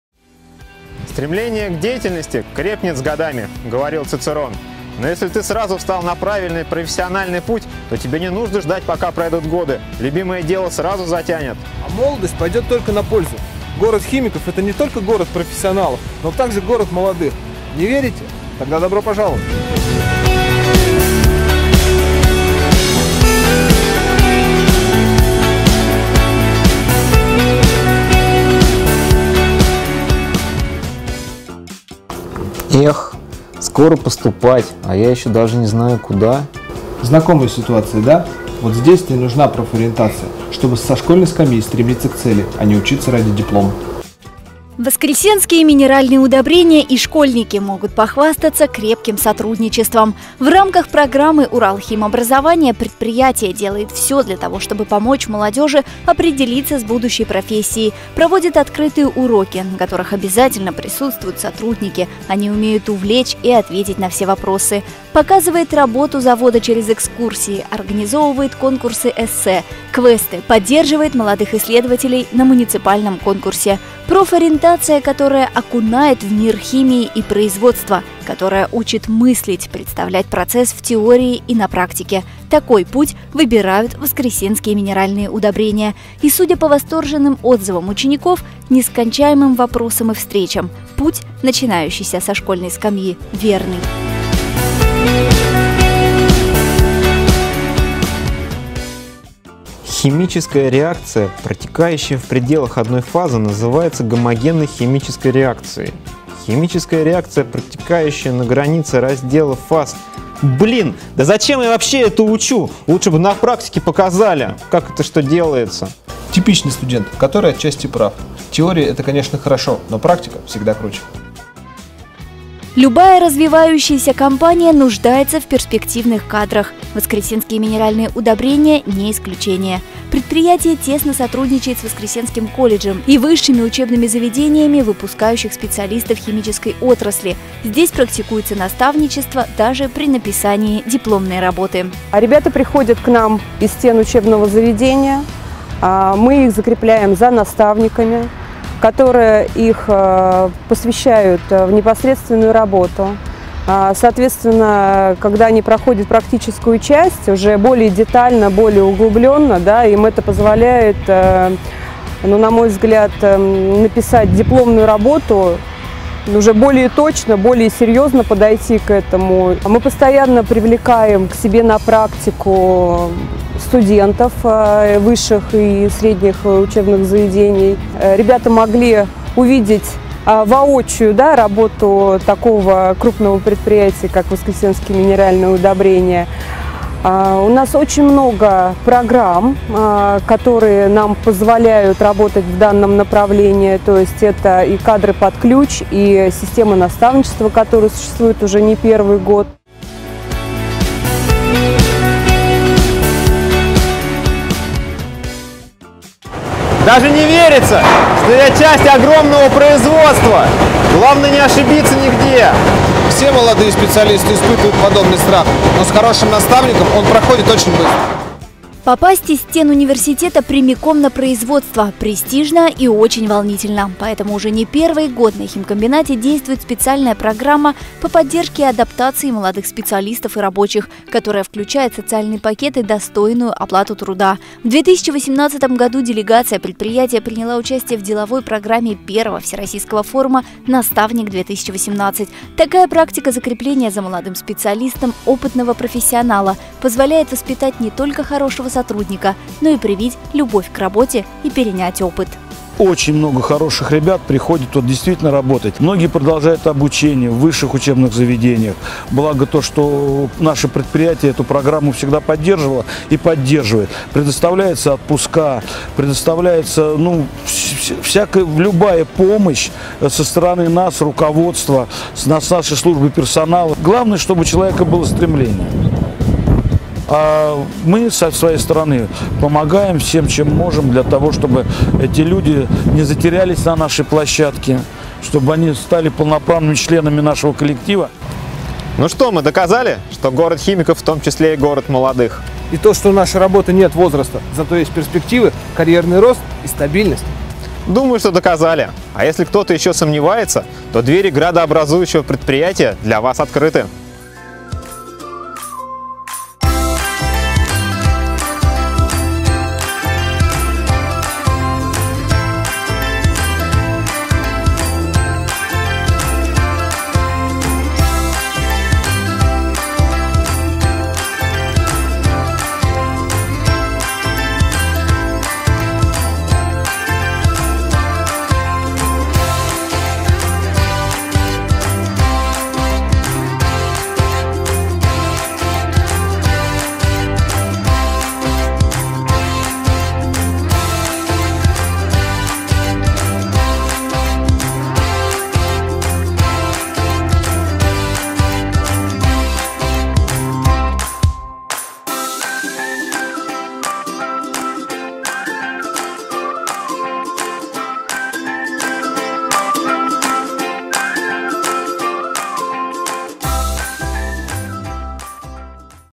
Воскресенск - город молодых! Специальный материал ко Дню химика
Градообразующее предприятие - это рабочие места для молодежи. О том, как начинающие специалисты находят свое призвание на заводе «Воскресенские минеральные удобрения», смотрите специальный материал.